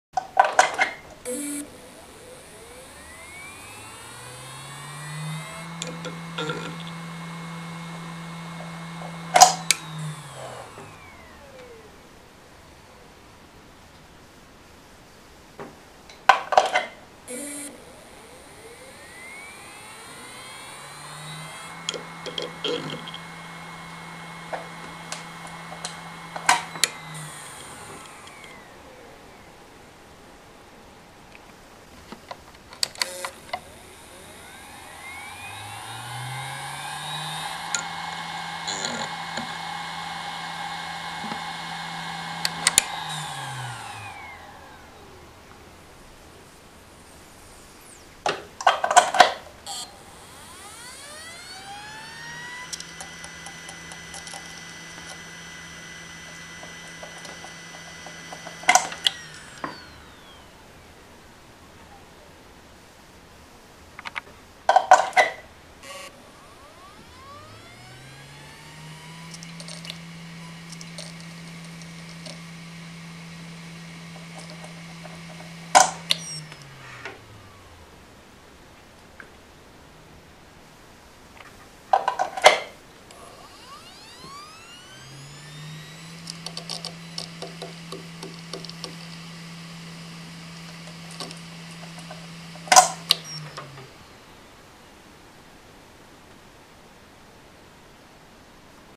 Western Digital Raptor/Velociraptor Generation 1-6 Spinup/Spindown sounds
Just a short clip to listen to all generations of WD Raptor
-WD360GD
-WD740GD
-WD3000GLFS
-WD1500AHFD
-WD6000HLHX
-WD1000DHTZ

Each drive is the top drive of its generation.
The WD-Raptor series are the only 10.000 RPM SATA drives on the market, even though they came out in 2003. But since SSDs are taking their place, i don´t think there wioll be a new generation.